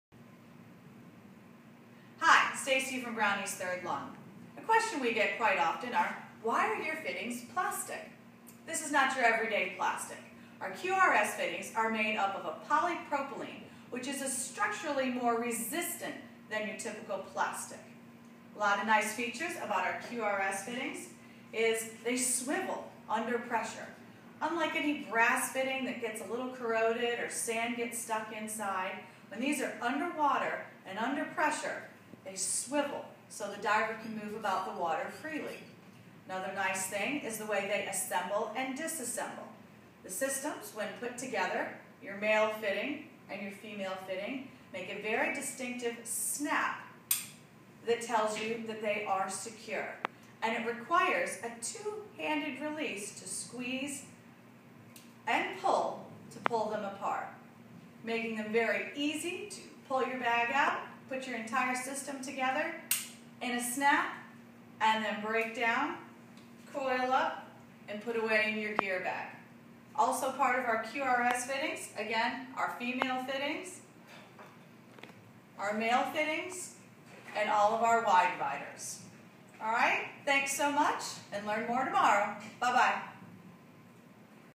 QRS Connectors Why Are You Using Plastic
Why do you use plastic for your connections?

One of the questions we at Brownie's Marine Group often get asked is why we use plastic for our connections - specifically for our Quick Release  (QRS).

Well, first of all as Stacy explains in this video, the material is not just any kind of plastic. But more than that they come with several benefits that are by far superior to other material choices.

Do not opt for the brass fittings because if you do you will lose out on these benefits.